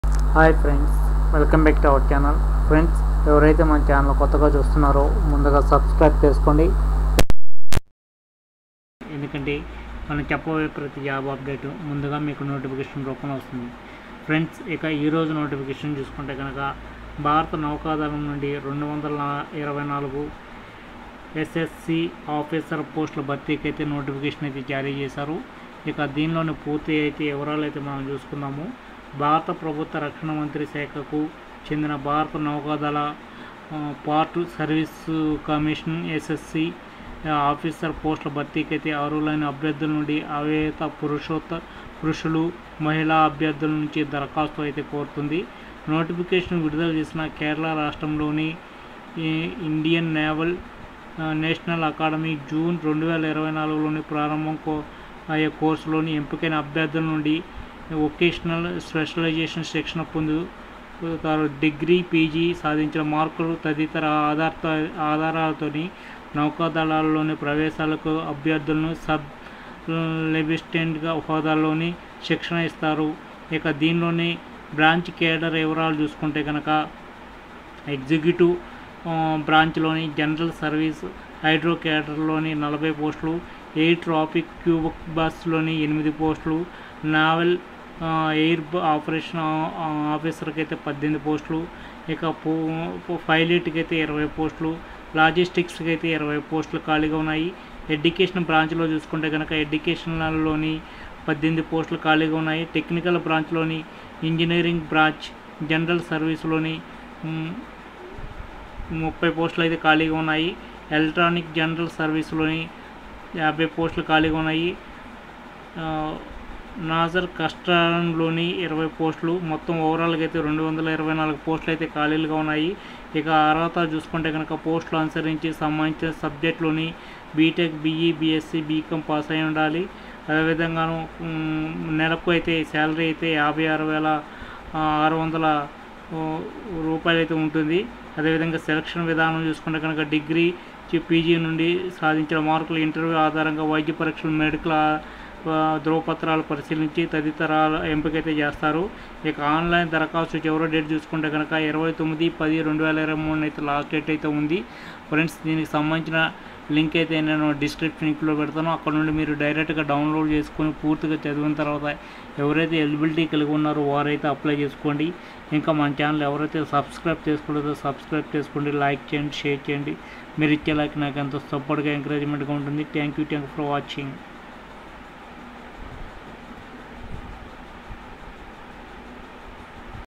Navy భారత నౌకాదళంలో 224 ఎస్ఎస్సి ఆఫీసర్ పోస్టులు
భారత ప్రభుత్వ రక్షణ మంత్రిత్వ శాఖకు చెందిన భారత నౌకాదళం... షార్ట్ సర్వీస్ కమిషన్ (ఎన్ఎన్ఎస్) ఆఫీసర్ పోస్టుల భర్తీకి అర్హులైన అవివాహిత పురుషులు, మహిళా అభ్యర్థుల నుంచి దరఖాస్తులు కోరుతూ నోటిఫికేషన్ విడుదల చేసింది. కేరళ రాష్ట్ర్యం ఎజిమలలోని ఇండియన్ నేవల్ అకాడమీ(ఐఎస్ఏ) జూన్ 2024లో ప్రారంభమయ్యే కోర్సులో ఎంపికైన అభ్యర్థులు సంబంధిత శాఖలు/ కేడర్/ స్పెషలైజేషన్లలో శిక్షణ పొందుతారు. డిగ్రీ, పీజీలో సాధించిన మార్కులు తదితరాల ఆధారంగా నౌకాదళంలో ప్రవేశాలుంటాయి. అభ్యర్థులు సబ్ లెఫ్టినెంట్ హోదాలో శిక్షణ ఉంటుంది. బ్రాంచి/ కేడర్ వివరాలు...
ఎగ్జిక్యూటివ్ బ్రాంచి:
1. జనరల్ సర్వీస్/హైడ్రో కేడర్: 60 పోస్టులు 2. ఎయిర్ ట్రాఫిక్ కంట్రోలర్: 08 పోస్టులు
3. నావల్ ఎయిర్ ఆపరేషన్స్ ఆఫీసర్: 18 పోస్టులు 4. పైలట్: 200 పోస్టులు
5. లాజిస్టిక్స్: 20 పోస్టులు
ఎడ్యుకేషన్ బ్రాంచి:
8. ఎడ్యుకేషన్: 18 పోస్టులు
7. ఇంజినీరింగ్ బ్రాంచి (జనరల్ సర్వీస్): 30 పోస్టులు 8. ఎలక్ట్రికల్ బ్రాంచి (జనరల్ సర్వీస్): 50 పోస్టులు
9. నావల్ కన్స్ట్రక్టర్: 20 పోస్టులు
మొత్తం ఖాళీల సంఖ్య: 224.
అర్హతలు: పోస్టును అనుసరించి సంబంధిత సబ్జెక్టులో బీటెక్, బీఈ, బీఎస్సీ, బీకాం, బీఎస్సీ(ఐటి), పీజీ డిప్లొమా, ఎంఎస్సీ, ఎంబీఏ, ఎంసీఏ, ఎంఎస్సీ (ఐటి), కమర్షియల్ పైలెట్ లైసెన్స్ ఉత్తీర్ణతతో పాటు నిర్దేశిత శారీరక ప్రమాణాలు ఉండాలి.
ప్రారంభ వేతనం: నెలకు రూ.56,100తో పాటు ఇతర అలవెన్సులు..
ఎంపిక విధానం: డిగ్రీ, పీజీలో సాధించిన మార్కులు, ఇంటర్వ్యూ, వైద్య పరీక్షలు, మెడికల్ స్టాండర్డ్స్, ధ్రువపత్రాల పరిశీలన తదితరాల ఆధారంగా.
ఆన్లైన్ దరఖాస్తుకు చివరి తేదీ: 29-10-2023.